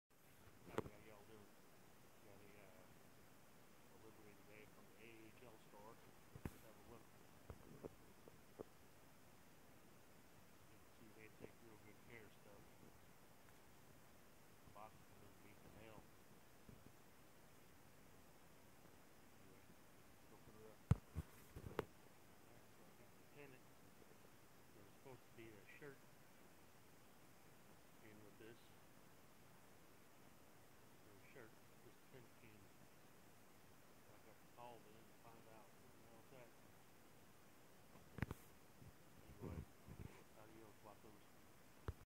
Hockey Unboxing
Audio in this is screwed up, microphone was full of dirt but if you use ear buds you can hear it lol